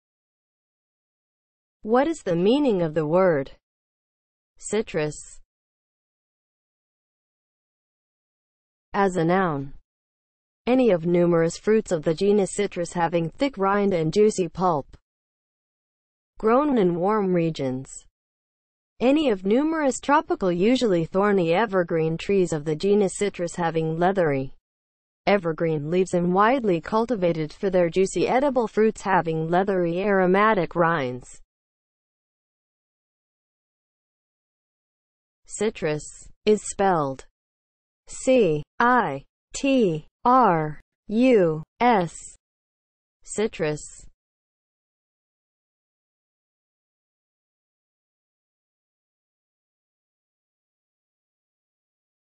What is the meaning of the word CITRUS?
Definition and spelling of the word CITRUS.

(00:00) Intro
(00:08) As a noun
(00:34) Spelling

As a noun (00:08):
• any of numerous fruits of the genus Citrus having thick rind and juicy pulp; grown in warm
  regions
• any of numerous tropical usually thorny evergreen trees of the genus Citrus having leathery
  evergreen leaves and widely cultivated for their juicy edible fruits having leathery aromatic
  rinds